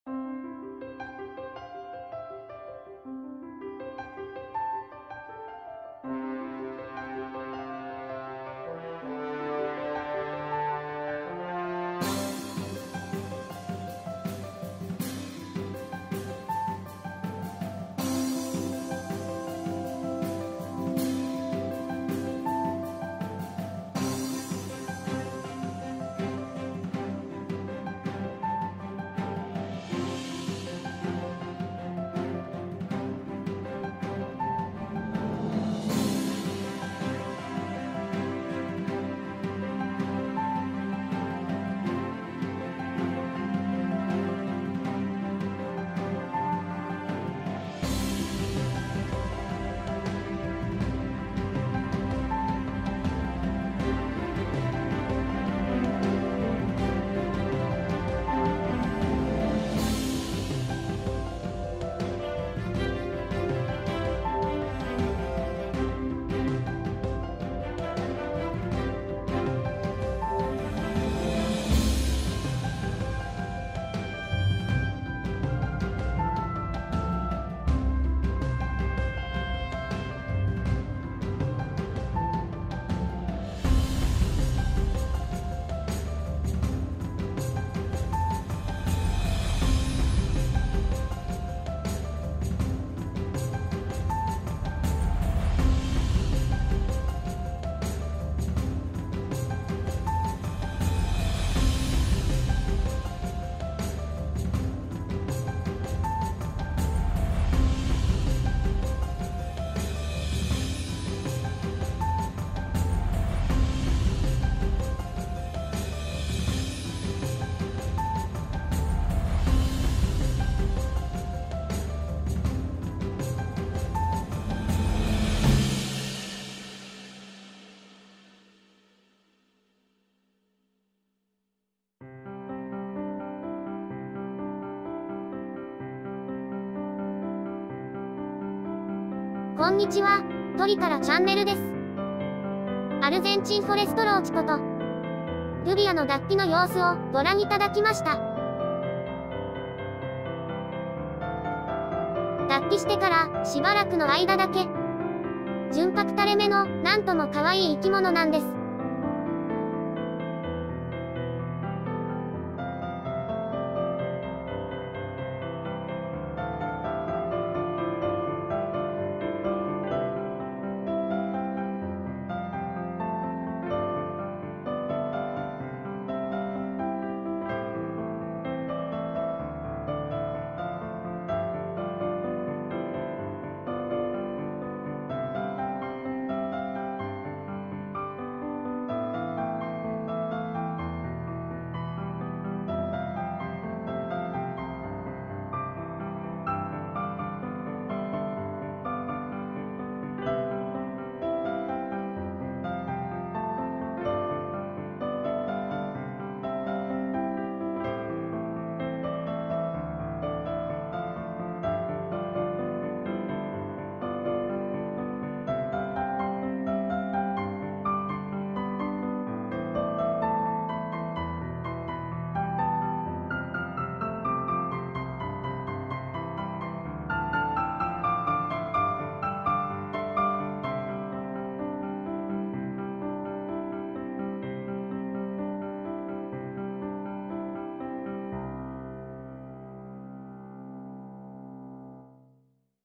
フェローチェ爆誕？【デュビア】
純白の御姿となる様子をお楽しみ下さい、デュビア脱皮の様子をおよそ15倍速でお送りしています。

ナレーション：CeVIO　さとうささら

＊2017年7月に公開しましたデュビア脱皮の動画と同じ映像素材を用いていますが、編集ソフトとPCを増強して今回は画質を向上させている・・つもりであります。